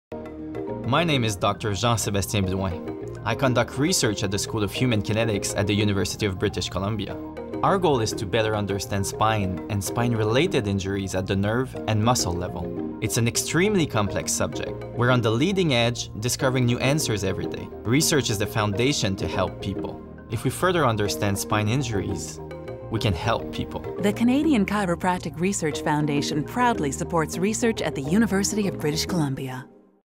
Dr. Jean-Sébastien Blouin - Better Understanding
Dr. Jean-Sébastien Blouin discusses his spine research at the University of British Columbia. The BC Chiropractic Association supports research on spine and spine related injuries.